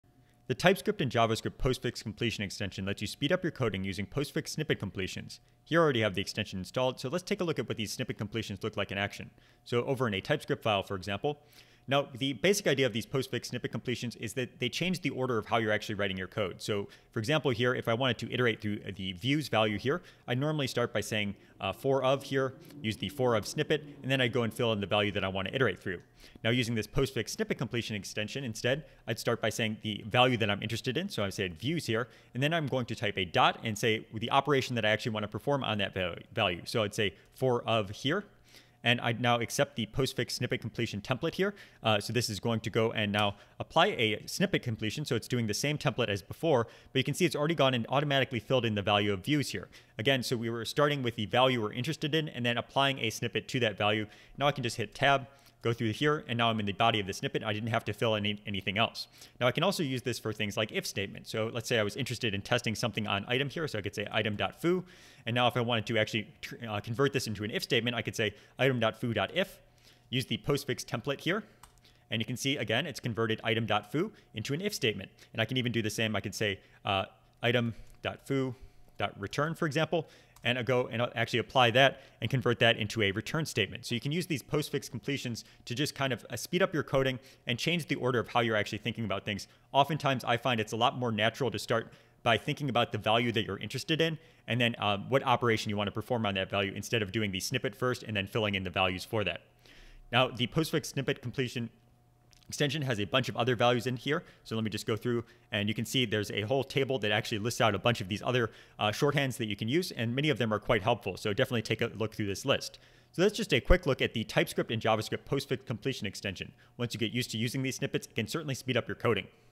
VS Code tips — TS/JS Postfix Completions
Today's VS Code extension: TS/JS Postfix Completions

Speed up your JavaScript/TypeScript coding using postfix completions

With postfix snippets, you write values first followed by what you want to do to the value, for example:

foo.if    becomes     if (foo) { }